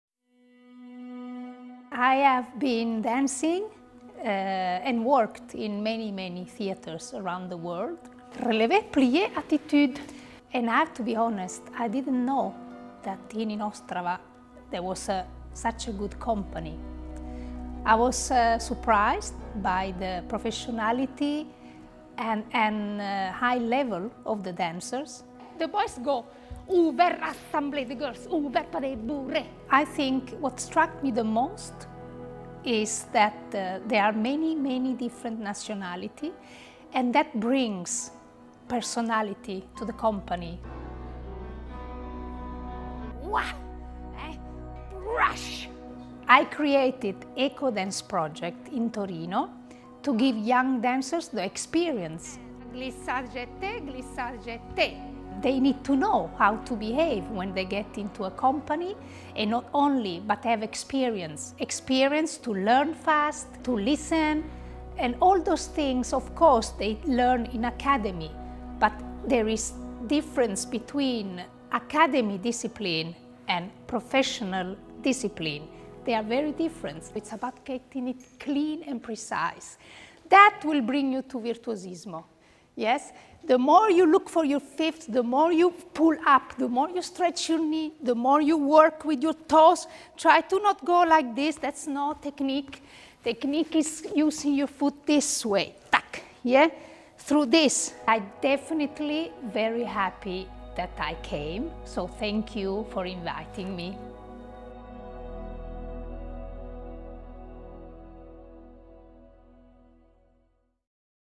Interview_Pompea_Santoro – Národní divadlo moravskoslezské
Interview s uznávanou pedagožkou Pompeou Santoro.
Videomaker: Marco Chiodi